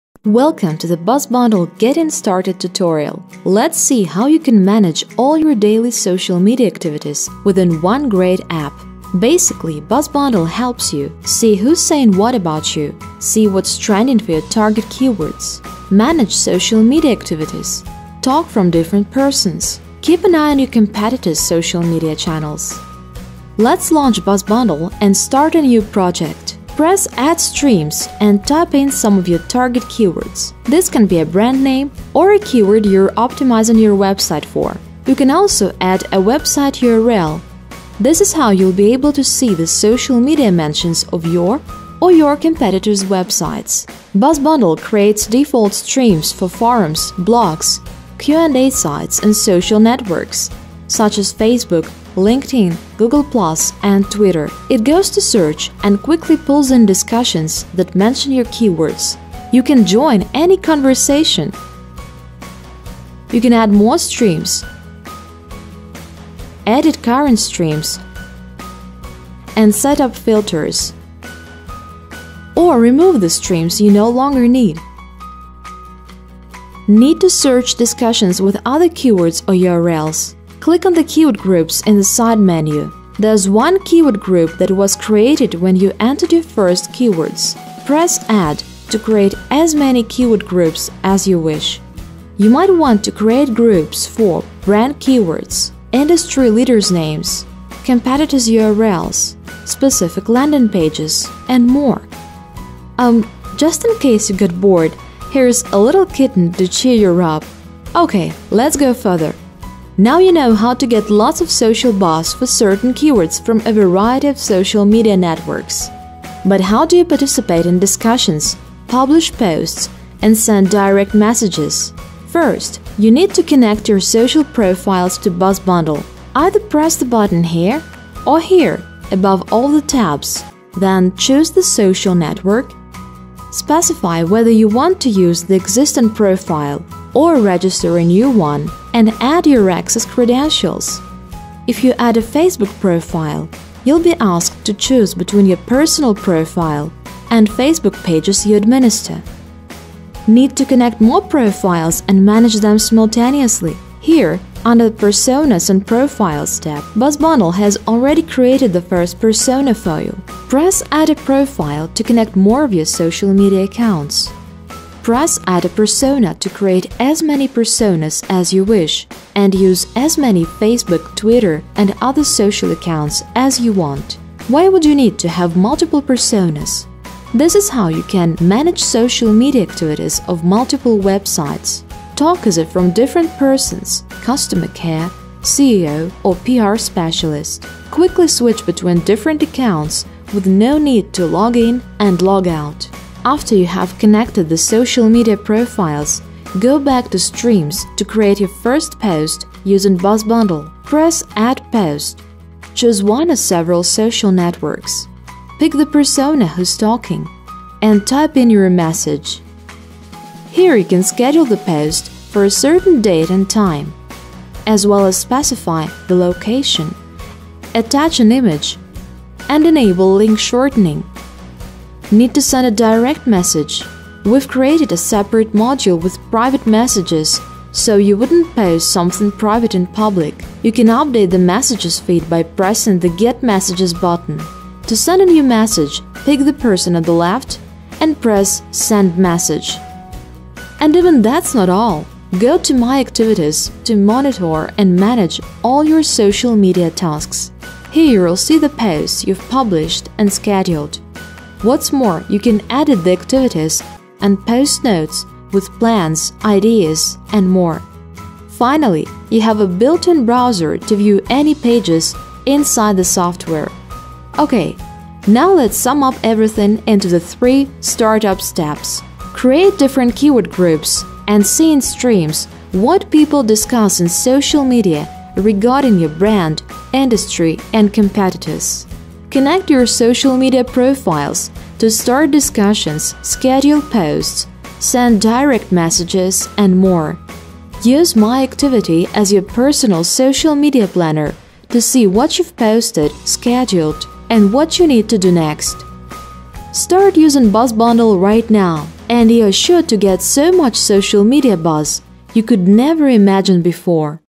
Getting started with Buzzbundle #1 social media monitor
Getting started tutorial with BuzzBundle the #1 Social Media management & monitoring tool. Covers Facebook, LinkedIn, Twitter, Google+ , plus every website, blog and forum that mentions you, your brand, your product. Find new customers and engage with them - find existing customers and hear what they are saying about you.